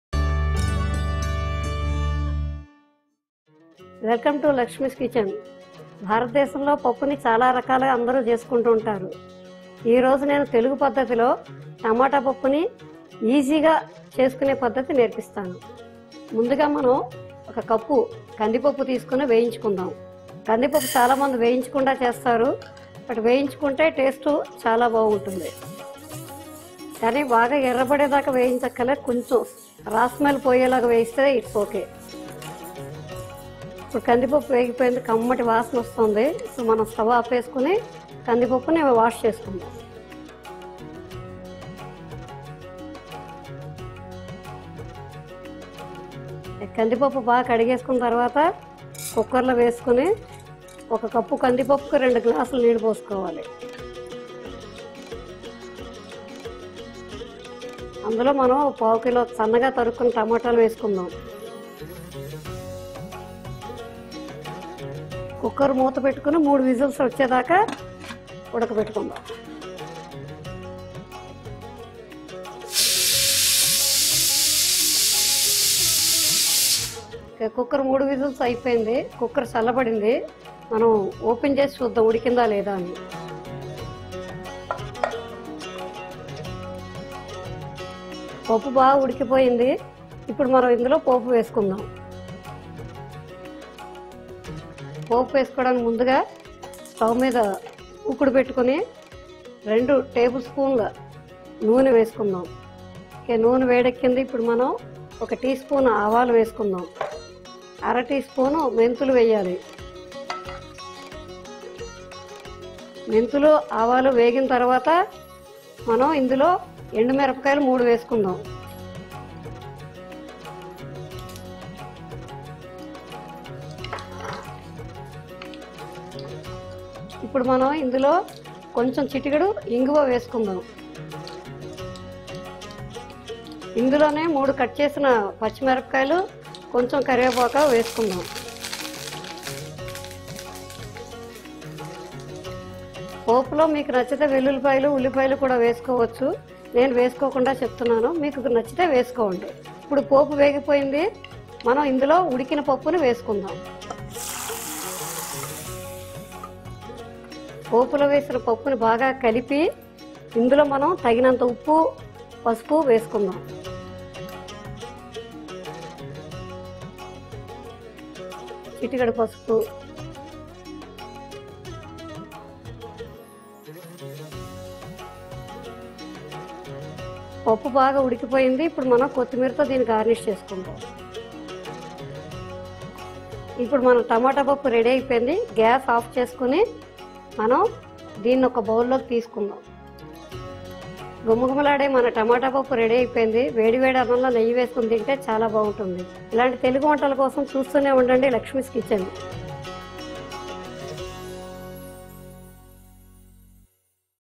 Tomato Dal Recipe | Andhra Style టమోటా పప్పు | South Indian Traditional Recipe | Lakshmi
Learn How To Make Andhra Style Tomato Dal Recipe From Our Chef Lakshmi Only On Variety Vantalu.

Make this simple, quick and easy popular Recipe of Tomato Dal at your home and share with us your experiences in the comments section below.

Ingredients:-
1 cup Pigeon Pea (Toor dal)(కందిపప్పు)
 250 grm Tomatoes
  Turmeric

For tempering:-
2 tbsp  Oil
1 tsp Mustard seeds
1/2 tsp  Fenugreek seeds
8-10 Curry Leaves
3 Red Chillies
Asafoetida
3 Slit Green Chillies
Coriander leaves

Method:-
-  Dry roast the Toor dal for about 4 to 5 minutes, Don't roast it too much, just roast it until it gives an aroma.
- Wash the Toor Dal.
- Add 250 grms Chopped Tomatoes and washed dal into the pressure cooker and cook them with 2 cups of water for about 3 whistles.
- Once Pressure cooker has cooled down, check the dal is cooked properly or not.
-For the final tempering, add oil and heat it.
- Once the oil is nice and hot add  Mustard seeds, Fenugreek seeds, Asafoetida, Red Chillies, Slit Green Chillies, Curry Leaves.
-Once it splutters pour the cooked dal into the pan.
-Mix it properly and add salt, turmeric.
-Garnish it with coriander leaves.

Host: Ramalakshmi
Director: Narayan Thakur
Camera: Kawaldeep Singh Jangwal, Spandan Rout, Akshay Sawant
Editing: Kishor Rai
Producer: Neha R. Barjatya
Copyrights: Rajshri Entertainment Private Limited